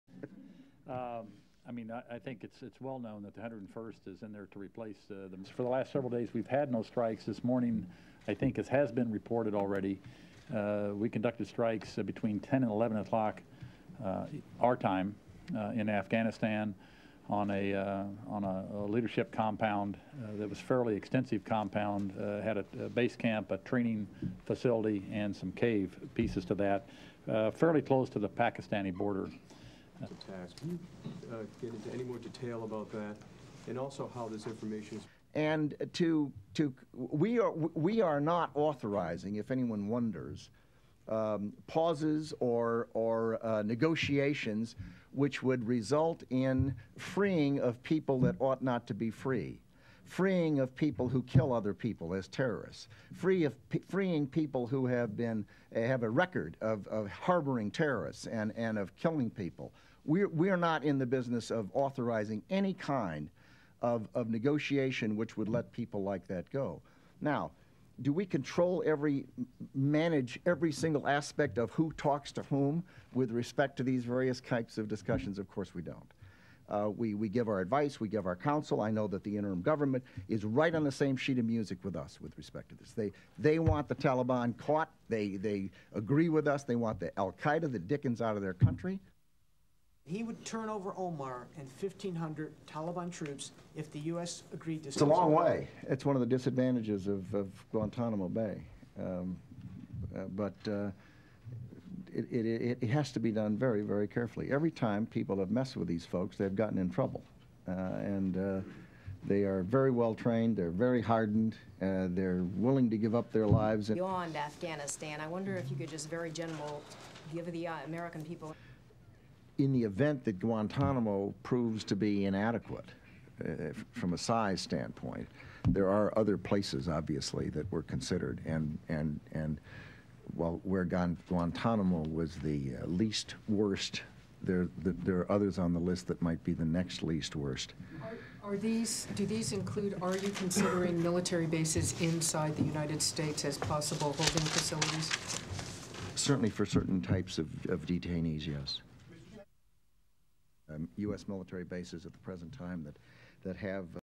US Defence Secretary and Gen Myers at Pentagon briefing
1. Wide shot General Richard Myers and Secretary Donald Rumsfeld at podium
2. SOUNDBITE: (English) General Richard Myers, Joint Chief of Staff Chairman
"For the last several days we've had no strikes. This morning, I think it has been reported already, we conducted strikes between 10 and 11 o'clock our time in Afghanistan on a leadership compound that was a fairly extensive compound, had a base camp, a training facility and some cave pieces to that, fairly close to the Pakistani border."
3. Cutaway Pentagon officials
4. SOUNDBITE: (English) Donald Rumsfeld, Secretary of Defence
"We are not authorizing, if anyone wonders, pauses or negotiations which would result in freeing of people that ought not to be freed, freeing of people who kill other people as terrorists, freeing people who have a record of harbouring terrorists and of killing people.  We are not in the business of authorizing any kind of negotiation which would let people like that go. Now, do we control every, manage every single aspect of who talks to whom with respect to these various types of discussions? Of course we don't. We give our advice, we give our counsel. I know the interim government is right on the same sheet of music with us with respect to this. They want the Taliban caught. They agree with us, they want the al-Qaida the dickens out of their country."
5. Cutaway reporters
6. SOUNDBITE: (English) Donald Rumsfeld, Secretary of Defence
"It's a long way. That's one of the disadvantages of Guantanamo Bay. But it has to be done very very carefully. Every time people have messed with these folks they've gotten in trouble. They are very well trained, they are very hardened. They're willing to give up their lives."
7. Cutaway
8. SOUNDBITE: (English) Donald Rumsfeld, Secretary of Defence
"In the event that Guantanamo proves to be inadequate, from a size standpoint, there are other places obviously that were considered. And, where Guantanamo was the least worst, there are others on the list that might be the next least worst."
(Question: Do these include - are you considering military bases inside the United States as possible holding facilities?)
"Certainly for certain types of detainees, yes."
9. Wide shot Myers and Rumsfeld

STORYLINE:

On Thursday, Air Force and Navy warplanes attacked a military compound south of Tora Bora in Afghanistan, which included a terrorist training camp and caves near the Pakistani  border.

General Richard Myers, Chairman of the Joint Chiefs of Staff, announced the strike at a Pentagon briefing conducted by himself and Defence Secretary Donald Rumsfeld.

Air strikes in Afghanistan have slowed in recent weeks.

The compound which was bombed on Thursday had been struck in 1998 by US cruise missiles but al-Qaida forces were still active there, officials said.

Secretary Rumsfeld defended the on-going hunt for al-Qaida leader Osama bin Laden and Mullah Mohammed Omar, the head of the defeated Taliban.

Rumsfeld pledged to bring the terrorist leader and his cohorts to justice, and described the US effort so far as successful, given that Taliban rule in Afghanistan has ended, and considerable disruption has been caused to the worldwide al-Qaida network.

But Rumsfeld said the war against terrorism is far from over, and that the hunt for those held responsible for the September 11 terrorist attacks against the United States will not end even if bin Laden and his top lieutenants are found.